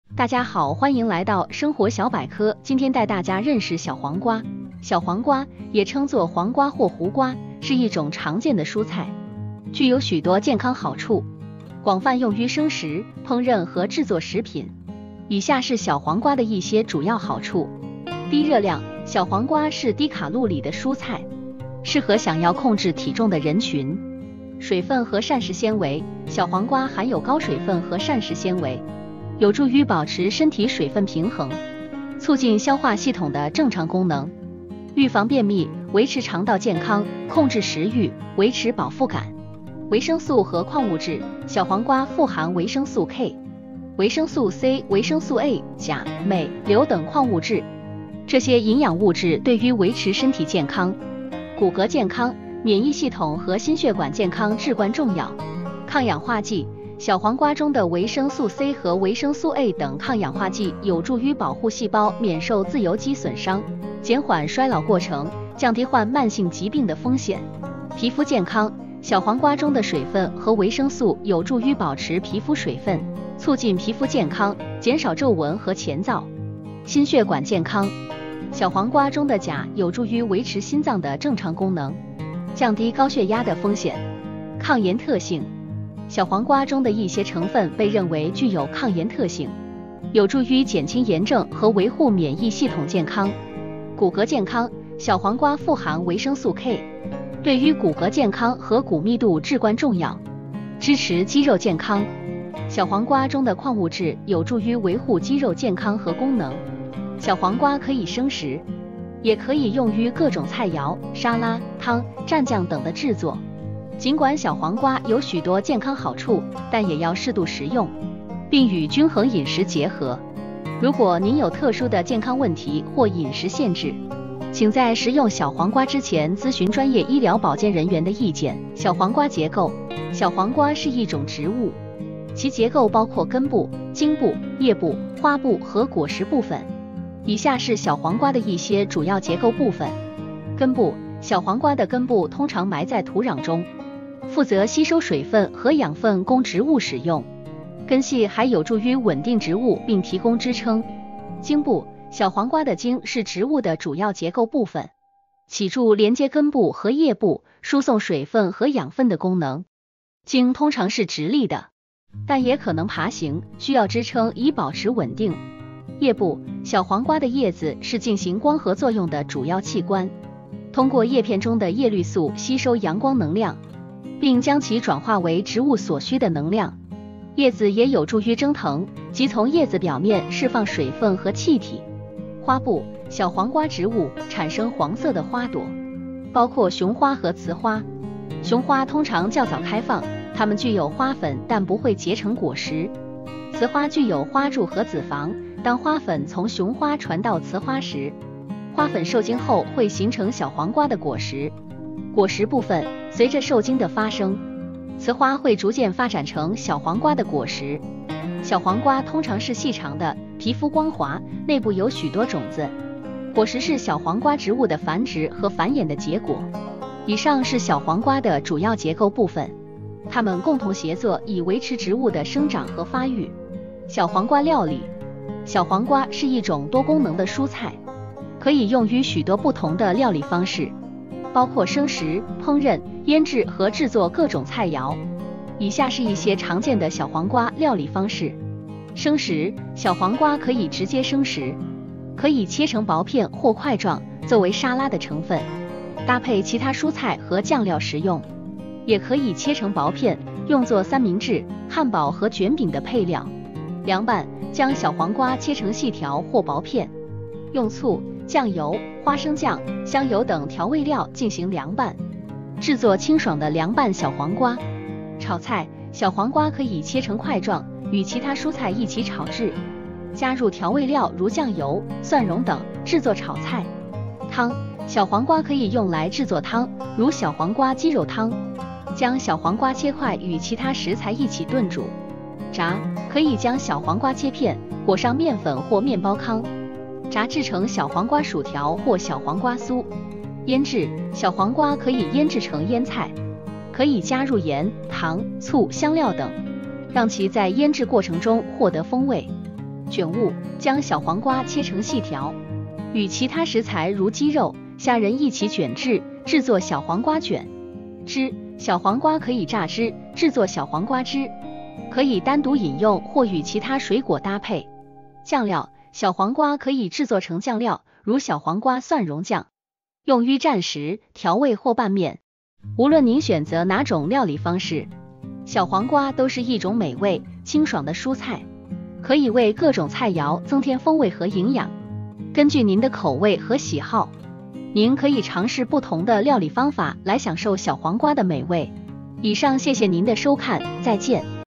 小黃瓜
小黃瓜，也稱作黃瓜或胡瓜，是一種常見的蔬菜，具有許多健康好處，廣泛用於生食、烹飪和製作食品。以下是小黃瓜的一些主要好處：

低熱量： 小黃瓜是低卡路里的蔬菜，適合想要控制體重的人群。

水分和膳食纖維： 小黃瓜含有高水分和膳食纖維，有助於保持身體水分平衡，促進消化系統的正常功能，預防便秘，維持腸道健康，控制食慾，維持飽腹感。

維生素和礦物質： 小黃瓜富含維生素 K、維生素 C、維生素 A、鉀、鎂、硫等礦物質。這些營養物質對於維持身體健康、骨骼健康、免疫系統和心血管健康至關重要。

抗氧化劑： 小黃瓜中的維生素 C 和維生素 A 等抗氧化劑有助於保護細胞免受自由基損傷，減緩衰老過程，降低患慢性疾病的風險。

皮膚健康： 小黃瓜中的水分和維生素有助於保持皮膚水分，促進皮膚健康，減少皺紋和乾燥。

心血管健康： 小黃瓜中的鉀有助於維持心臟的正常功能，降低高血壓的風險。

抗炎特性： 小黃瓜中的一些成分被認為具有抗炎特性，有助於減輕炎症和維護免疫系統健康。

骨骼健康： 小黃瓜富含維生素 K，對於骨骼健康和骨密度至關重要。

支持肌肉健康： 小黃瓜中的礦物質有助於維護肌肉健康和功能。

小黃瓜可以生食，也可以用於各種菜餚、沙拉、湯、蘸醬等的製作。儘管小黃瓜有許多健康好處，但也要適度食用，並與均衡飲食結合。如果您有特殊的健康問題或飲食限制，請在食用小黃瓜之前諮詢專業醫療保健人員的意見。